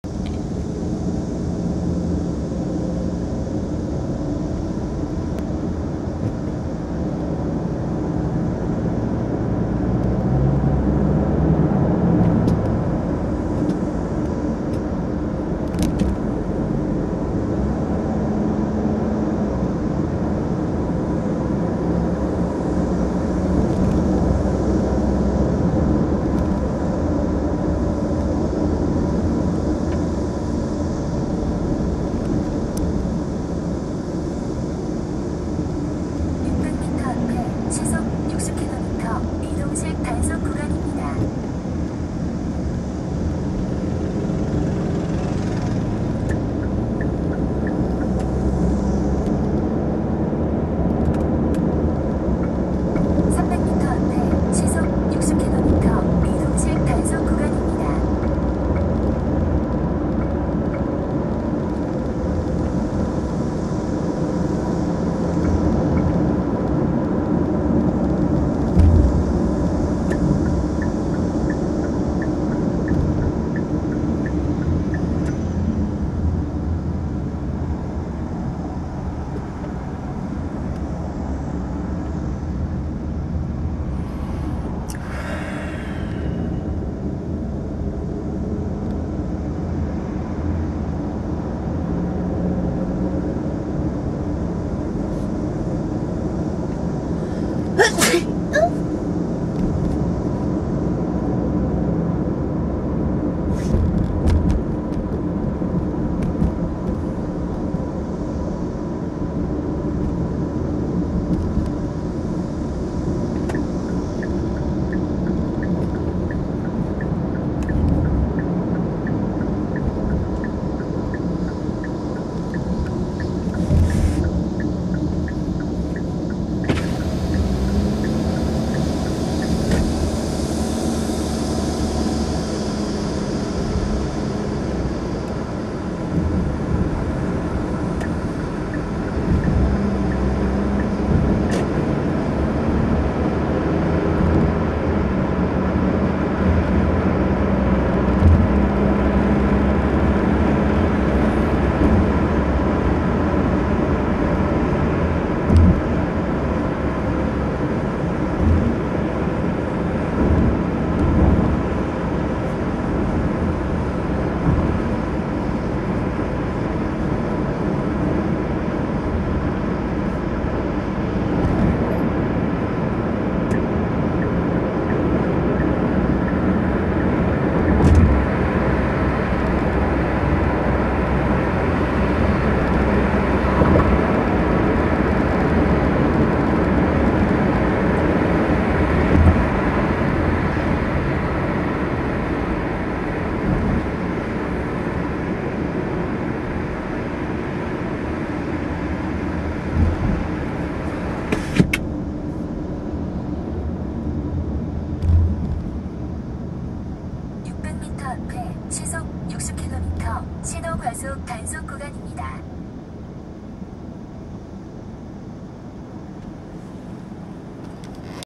JKP cTV 노들길 양화대교 복댠 nodeul Yanghwa BRDG
노들길 양화대교 북단(JKP cTV 노들길 양화대교 복댠 nodeul Yanghwa BRDG )

노들길 양화대교 북단으로 주행한 영상이다.